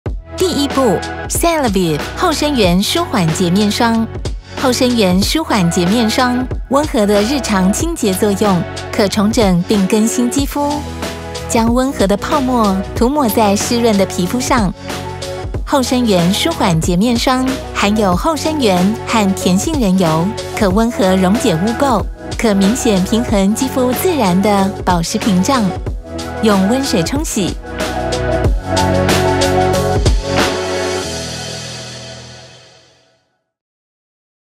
第一步：Celavive 後生元舒緩潔面霜
後生元的科學將保濕植物成分、神經酰胺和萃取油加入我們的  USANA Celavive 後生元護膚產品系列。

平衡的美容微生物群和微生物友好的成分能夠有效改善有問題的肌膚——我們推出了四款敏感肌適用的產品，為您的護膚程序添增舒緩保濕。

什麼是後生元？
後生元是益生菌發酵的副產物。

配方內含：
後生元乳酸菌發酵物：針對改善乾燥肌膚並補充肌膚水分屏障。舒緩潔面霜和活膚後生元保濕膏中含有此成分。
後生元異構寡糖：是海洋後生元，可有效平衡、強效保濕並舒緩肌膚。鎮靜調理精華中含有此成分。
後生元霍亞花花蜜、酵母提取物：為肌膚的保濕屏障提供補水。舒緩保濕霜中含有此成分。

後生元為您的護膚程序提供以下益處：
• 幫助明顯改善需要額外護理和支持補水的問題肌膚。
• 支持肌膚保濕屏障問題——如痘痘肌、乾斑和因乾燥引起的泛紅。
• 不含香精、刺激成分、對羥基苯甲酸酯和刺激性化學物質。

前往 USANA.com 瞭解詳情。

體驗 Celavive 微生物群友好的後生元護膚產品。